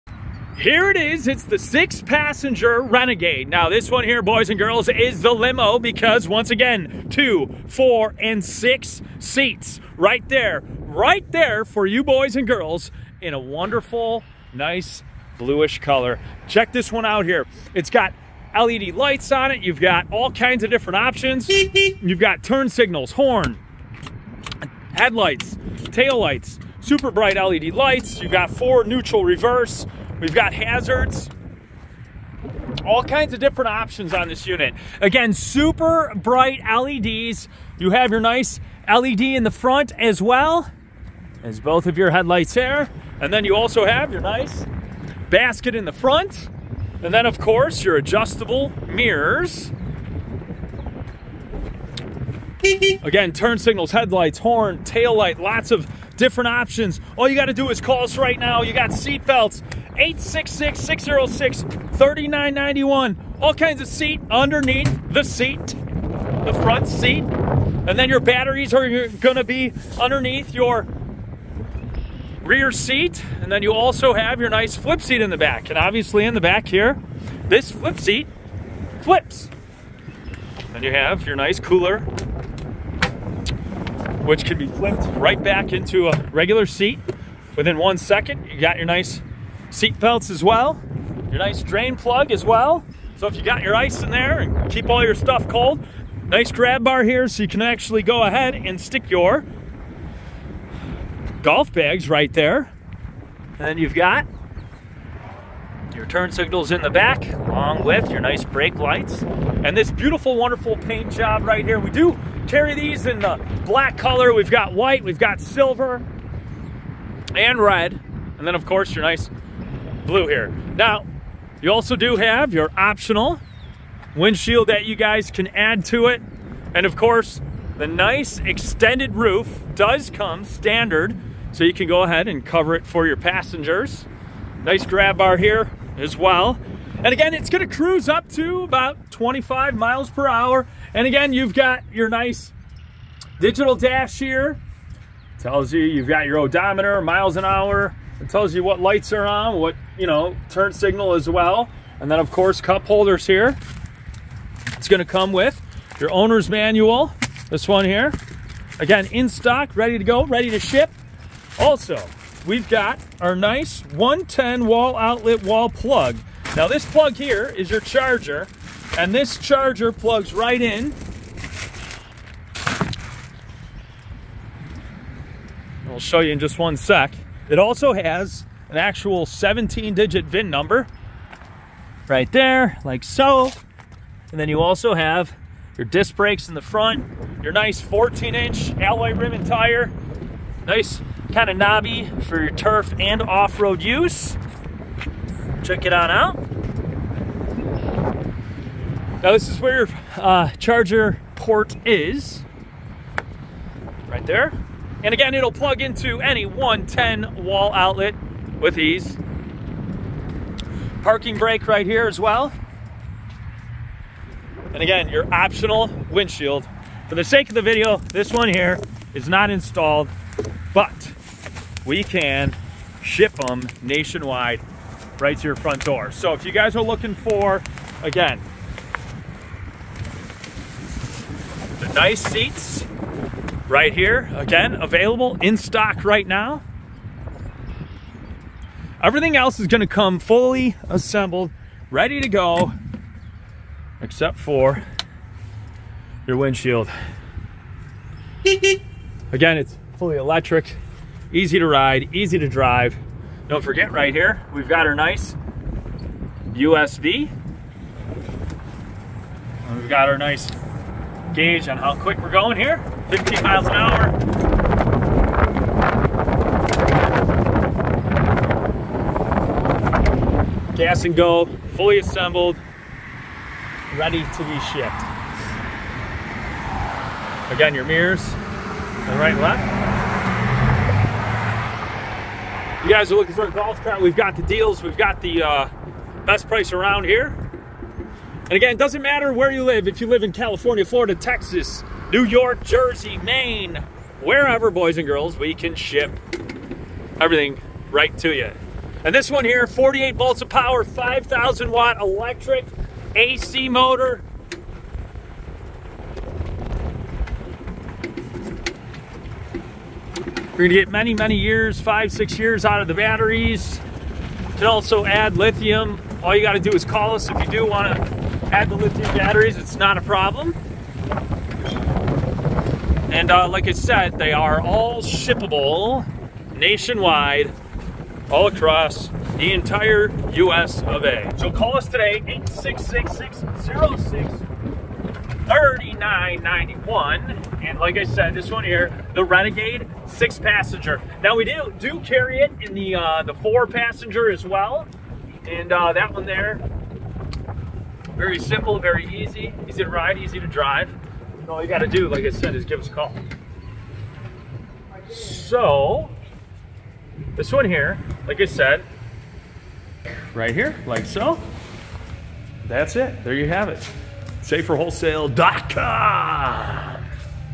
6 Passenger Renegade Limo Electric Golf Cart 48v 5000 Watt From SaferWholesale
Check Out The 48v Electric Renegade Golf Cart In Stock Now From SaferWholesale. We Carry Many Colors With Lots Of Available Options! For More Information And Pricing!